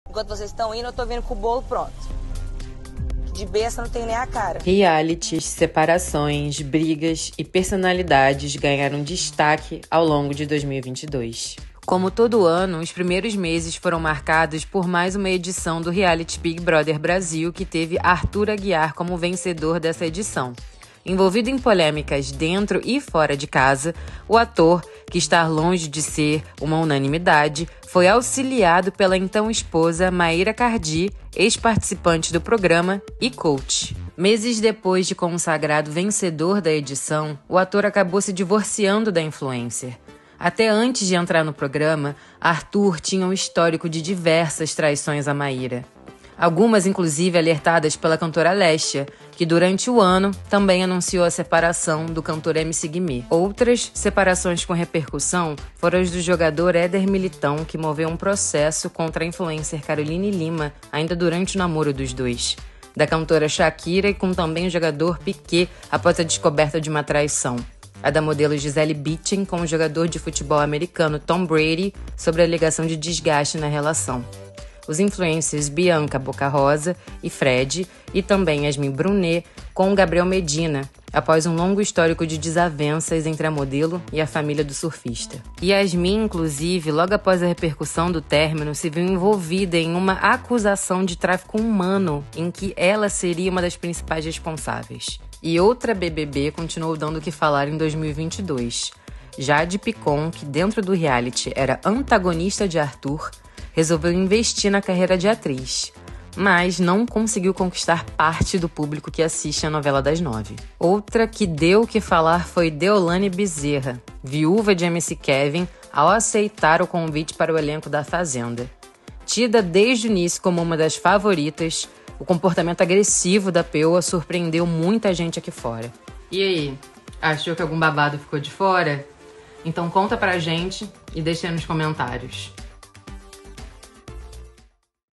Retrospectiva das Celebs: Ganhou o reality, perdeu a esposa 🍞📺
As celebridades deram o que falar em 2022. Vamos relembrar algumas das principais notícias envolvendo influencers, atletas e artistas durante o ano.

🎥 Reprodução/ TV Globo